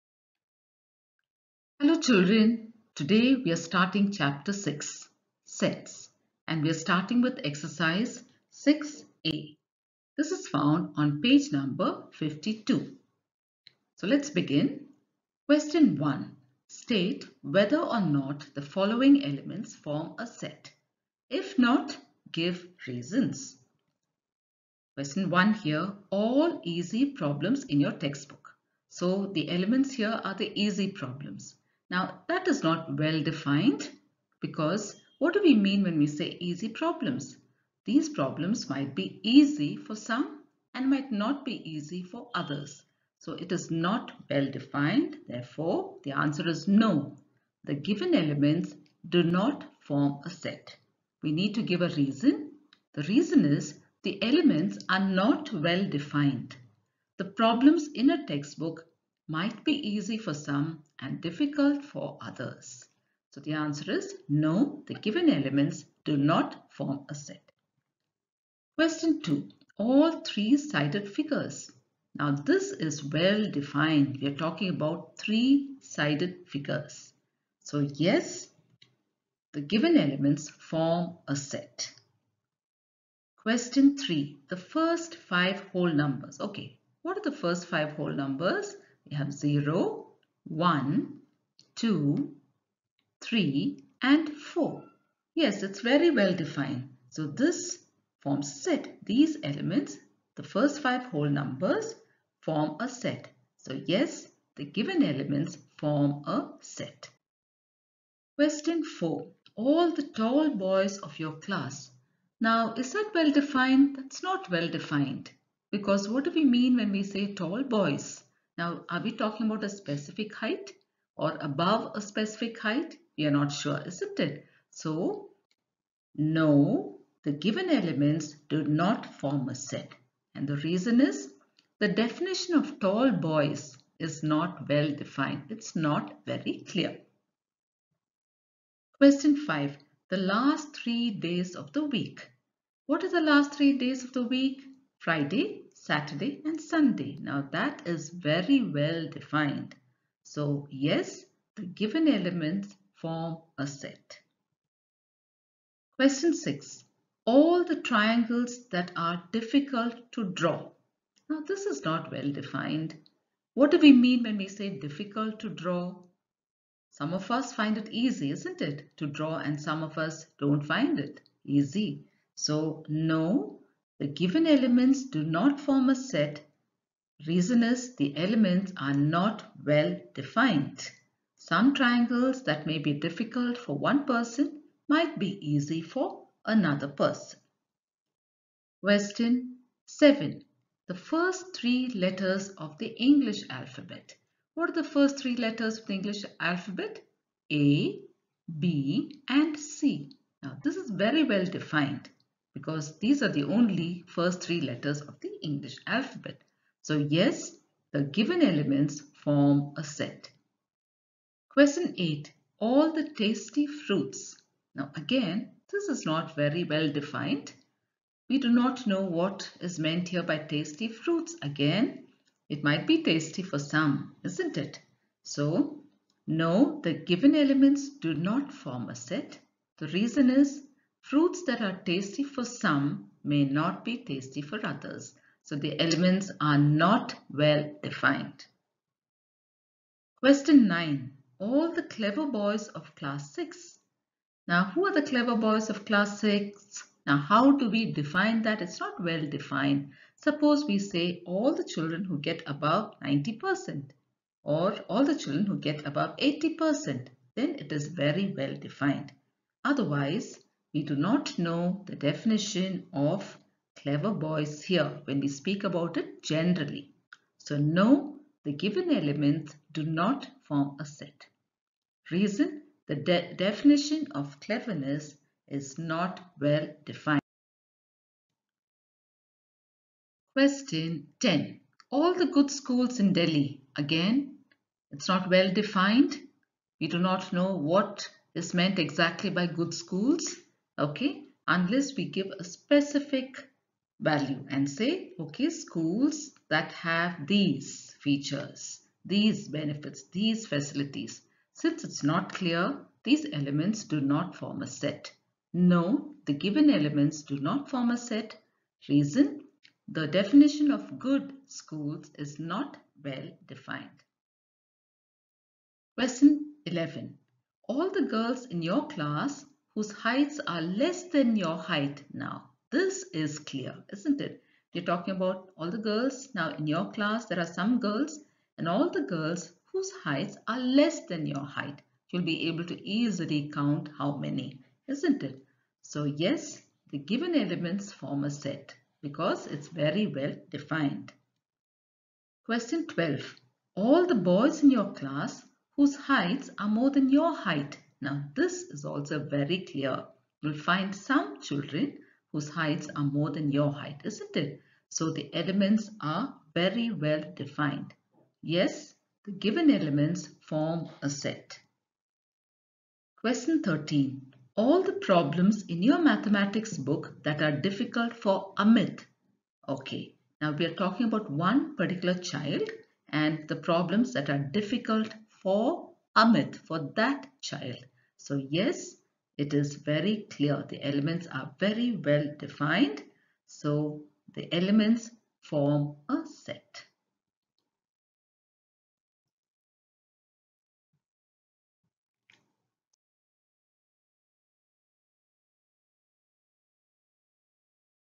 SETS - CHAPTER 6, EXERCISE 6(A)
Hi students. We are following Concise Mathematics for Middle School - Class 6, by Selina Publishers. This is the revised edition - November 2021.We are doing Chapter 6 - SETS, Exercise 6(A)

Sets.
Idea of a set.
Well defined sets.